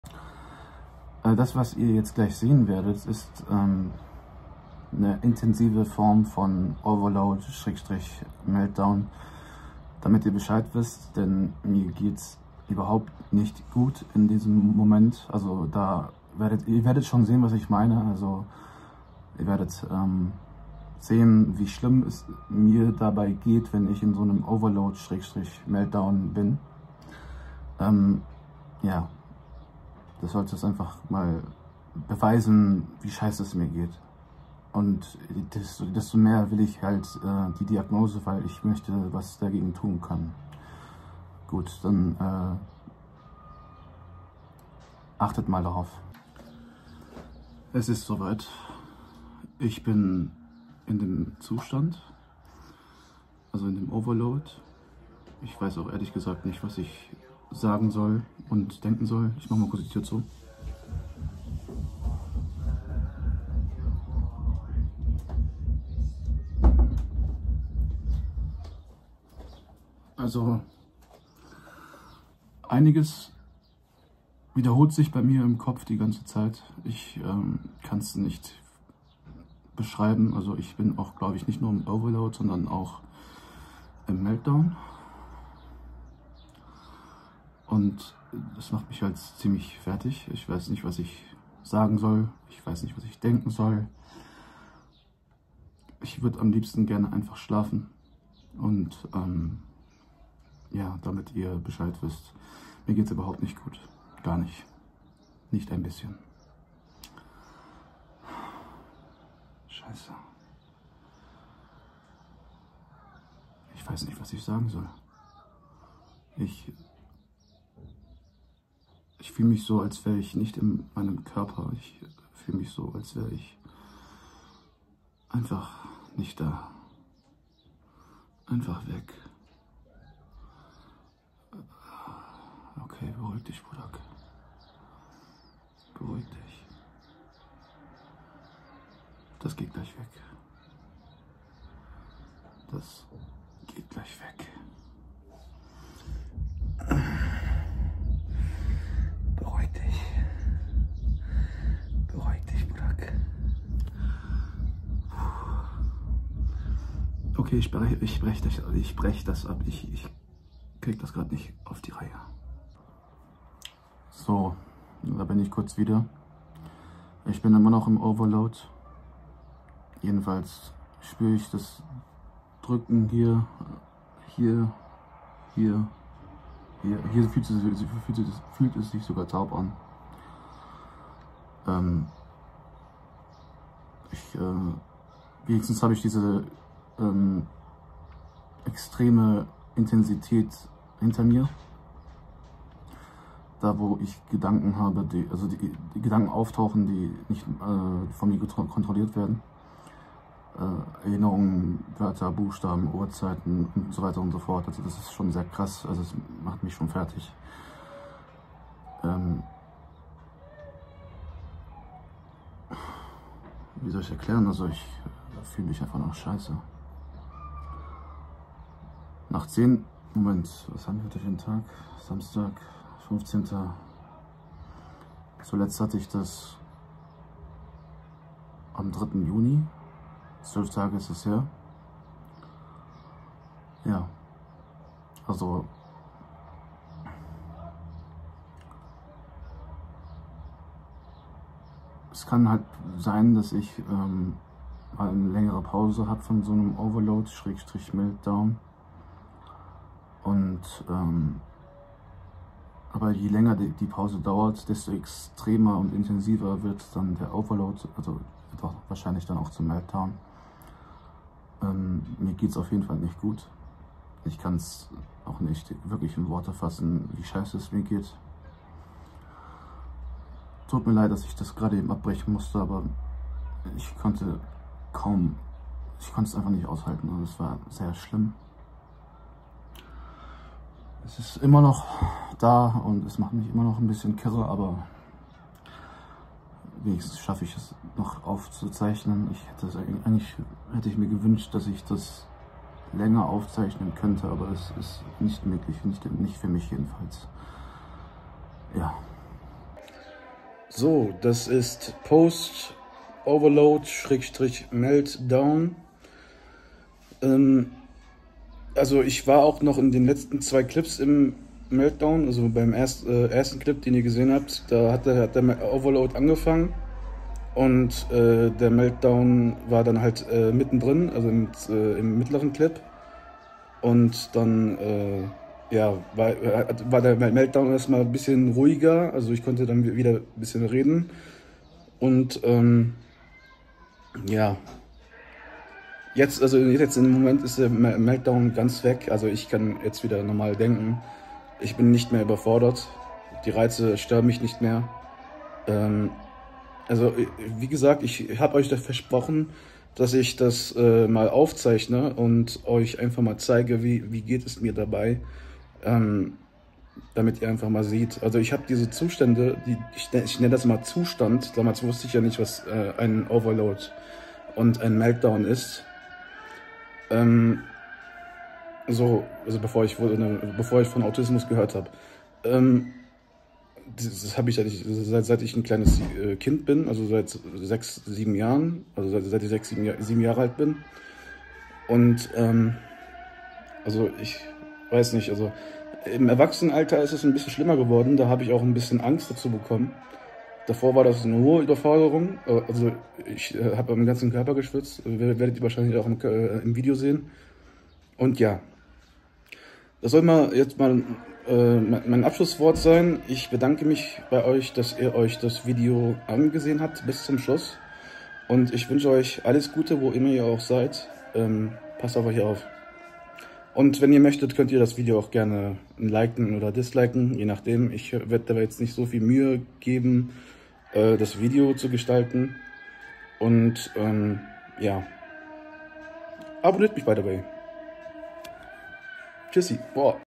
Mein Overload und Meltdown aufgezeichnet
Direkt als der Overload begann und ich Zuhause war, nahm ich mein Handy und zeichnete es auf. Ihr müsst wissen, wenn ich in so einem Overload bin, fallen mir die leichtesten Dinge extrem schwer.